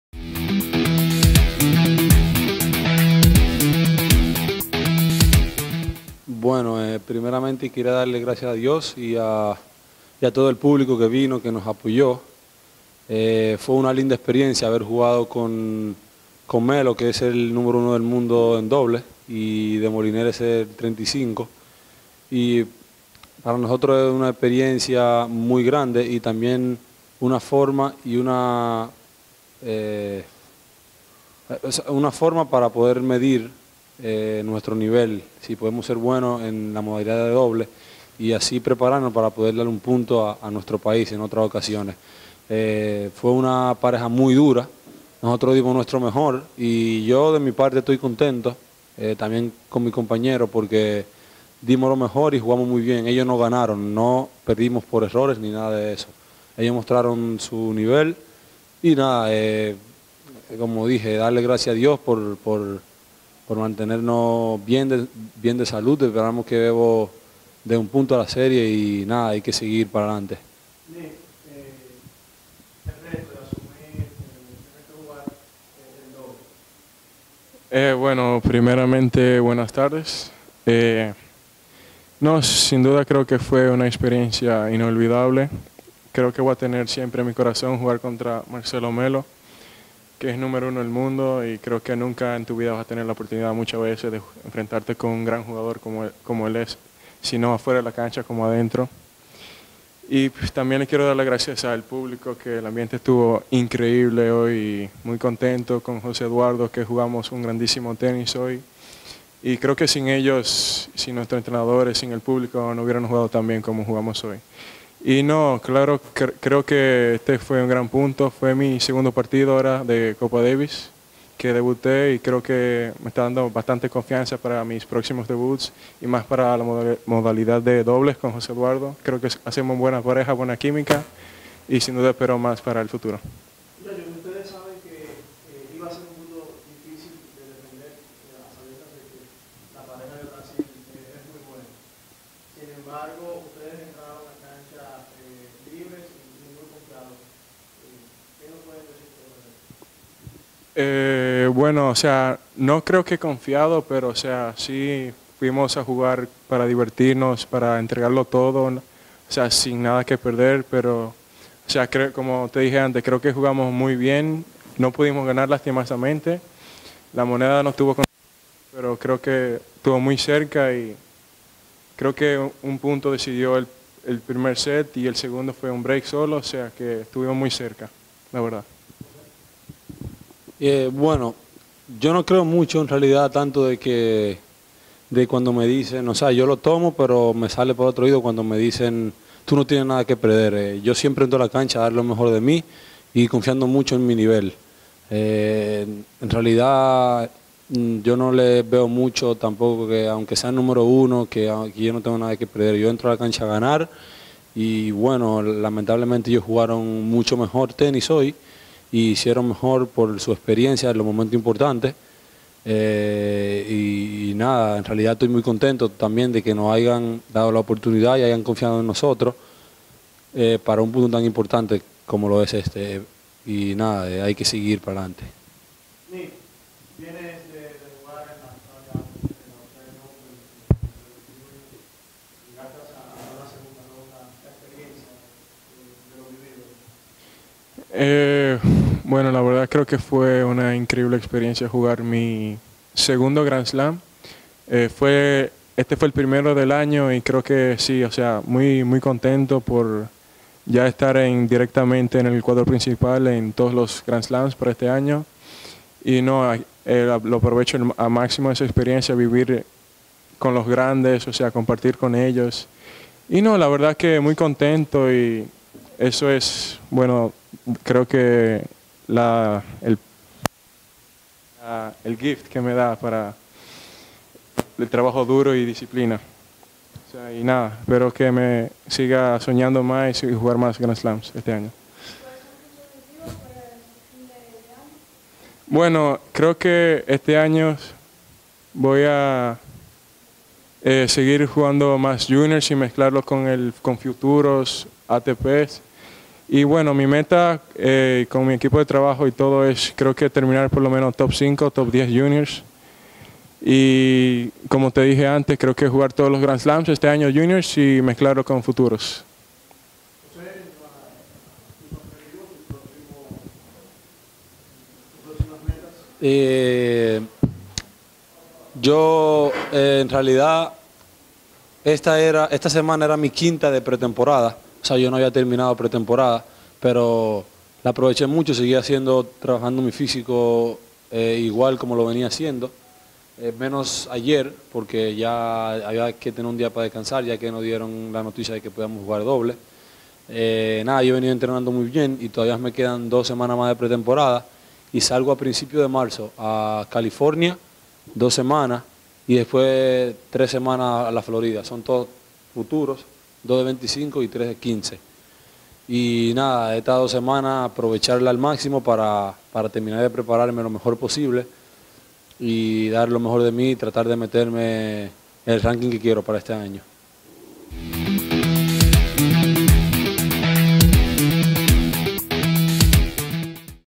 Nick Hardt y José Olivares - Copa Davis DOM vs BRA (3-2-2018)
Hardt y Olivares hablan de la experiencia de jugar frente a Marcelo Melo número 1 en dobles ATP.